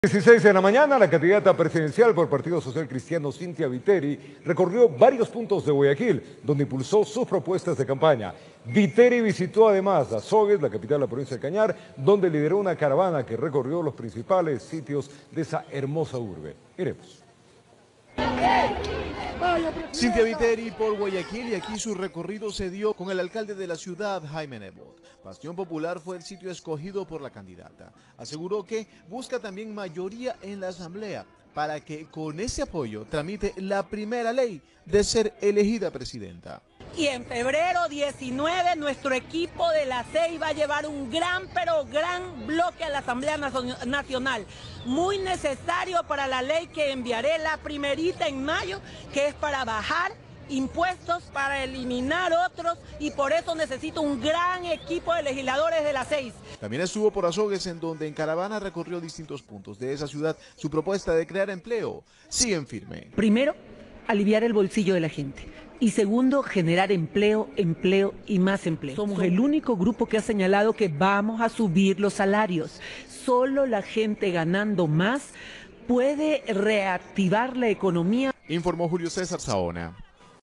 Cynthia Viteri recorrió Guayaquil y Azogues
La candidata por PSC en compañía del alcalde de Guayaquil recorrió la ciudad y fue enfática en su propuesta de aumentar el sueldo de los ecuatorianos. Continúa recorriendo el país